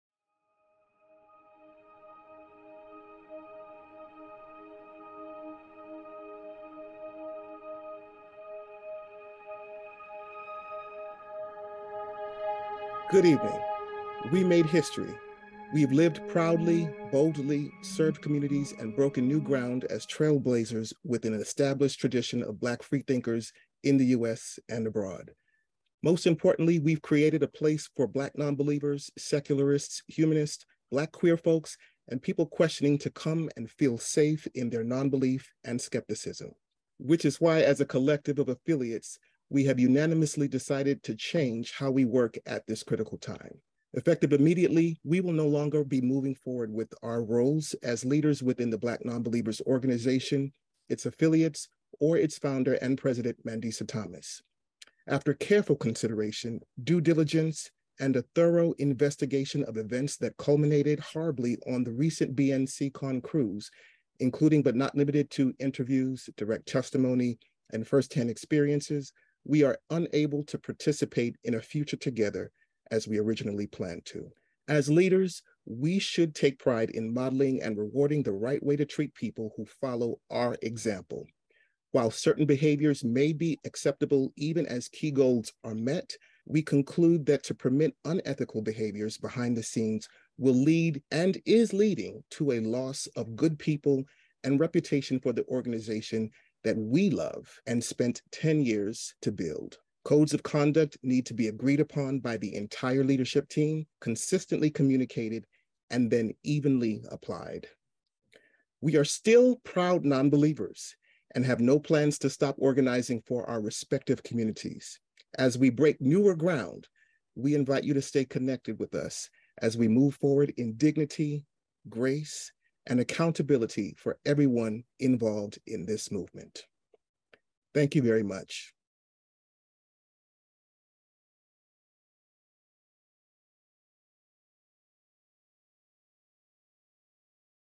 Black Nonbelievers - Leadership Resignation Announcement (Dec 2022)
An investigation into malfeasance and ethics was conducted by the departing leadership, of which, collaborated in the production of three (3) Incident reports from three (3) regional directors. One director signed-on to the mass resignation, though she was not present for this video announcement.

The incident reports detail established dynamics of pattern and practice regarding the then Executive Director of Black Nonbelievers, from which Black Nonbelievers of DC (“BNDC”) derived its branding, mission statement and member relationship.

The departure of 5/6th's of BN's regional leadership (inc. Black Nonbelievers of DC, an “affiliate”) is predicated upon, but not limited to, the credible and demonstrable accounts detailed in the incident reports.

Click to view the incident reports from BNDC, BNLOU, BNNY (published Dec. 20th, 2022)